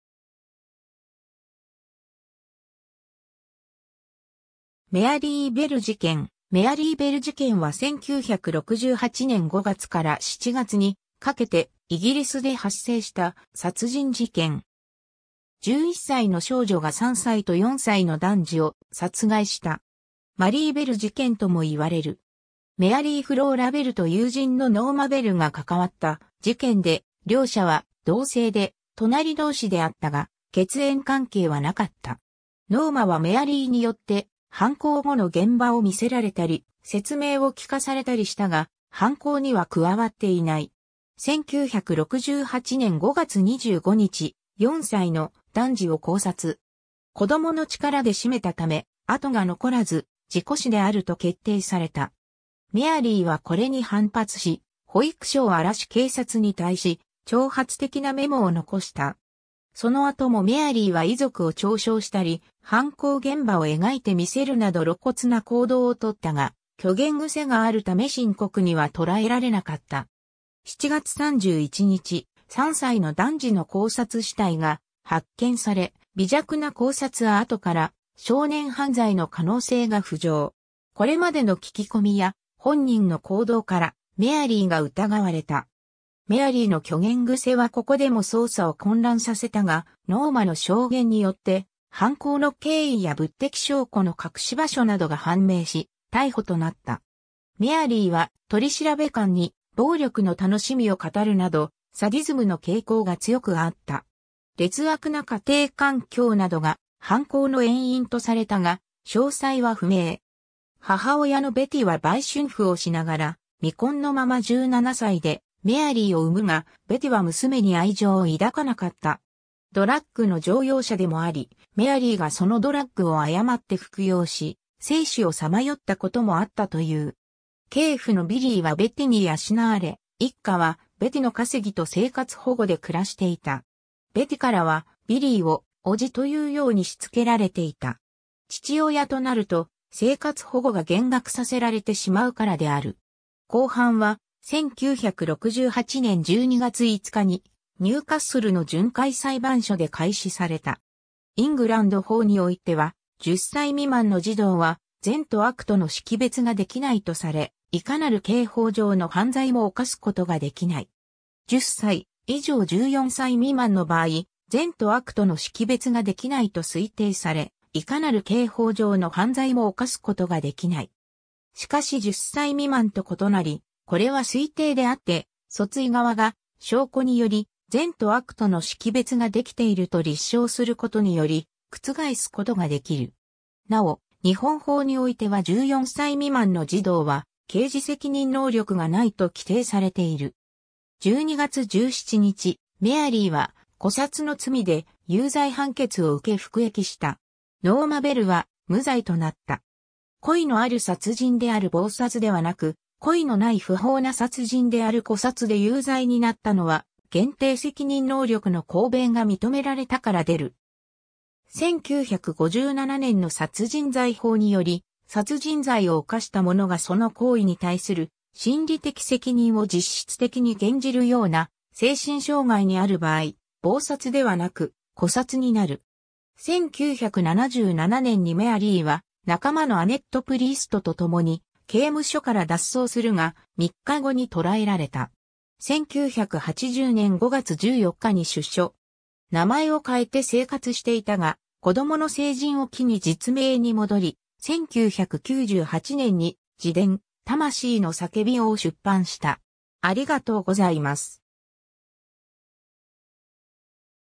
メアリー・ベル事件
ニューカッスル・アポン・タイン
メアリー・ベル事件 メアリー・ベル(Mary Bell)事件は1968年5月から7月にかけてイギリスで発生した殺人事件。
11歳（初犯は10歳）の少女が3歳と4歳の男児を殺害した。
マリー・ベル事件とも言われる。
 メアリー・フローラ・ベルと友人のノーマ・ベルが関わった事件で、両者は同姓で隣同士であったが血縁関係はなかった。
ノーマはメアリーによって犯行後の現場を見せられたり、説明を聞かされたりしたが、犯行には加わっていない。
 1968年5月25日、4歳の男児を絞殺。
子供の力で絞めたため跡が残らず、事故死であると決定された。
メアリーはこれに反発し、保育所を荒らし警察に対し挑発的なメモを残した。
その後もメアリーは遺族を嘲笑したり、犯行現場を描いてみせるなど露骨な行動を取ったが、虚言癖があるため深刻には捉えられなかった。
7月31日、3歳の男児の絞殺死体が発見され、微弱な絞殺跡から少年犯罪の可能性が浮上。
これまでの聞き込みや本人の行動から、メアリーが疑われた。
メアリーの虚言癖はここでも捜査を混乱させたが、ノーマの証言(当初は関与を否定していた)によって犯行の経緯や物的証拠の隠し場所などが判明し、逮捕となった。
 メアリーは取調官に暴力の楽しみを語るなど、サディズムの傾向が強くあった。
劣悪な家庭環境などが犯行の遠因とされたが、詳細は不明。
母親のベティは売春婦をしながら未婚のまま17歳でメアリーを産むが、ベティは娘に愛情を抱かなかった。
ドラッグの常用者でもあり、メアリーがそのドラッグを誤って服用し、生死をさまよったこともあったという。
継父のビリーはベティに養われ、一家はベティの稼ぎと生活保護で暮らしていた。
ベティからは、ビリーを「叔父」というように躾けられていた。
父親となると、生活保護が減額させられてしまうからである。
 公判は、1968年12月5日にニューカッスルの巡回裁判所で開始された。
イングランド法においては、10歳未満の児童は善と悪との識別ができないとされ、いかなる刑法上の犯罪も犯すことができない。
10歳以上14歳未満の場合、善と悪との識別ができないと推定され、いかなる刑法上の犯罪も犯すことができない。
しかし10歳未満と異なり、これは推定であって訴追側が証拠により善と悪との識別ができていると立証することにより覆すことができる。
なお日本法においては14歳未満の児童は刑事責任能力がないと規定されている。
12月17日、メアリーは故殺の罪で有罪判決を受け服役した。
ノーマ・ベルは無罪となった。
故意のある殺人である謀殺ではなく、故意のない不法な殺人である故殺で有罪になったのは限定責任能力の抗弁が認められたからでる。
1957年の殺人罪法により、殺人罪を犯した者がその行為に対する心理的責任を実質的に減じるような精神障害にある場合、謀殺ではなく故殺になる。
 1977年にメアリーは、仲間のアネット・プリーストと共に刑務所から脱走するが、3日後に捕らえられた。
1980年5月14日に出所。
名前を変えて生活していたが、子供の成人を機に実名に戻り、1998年に自伝『魂の叫び』を出版した。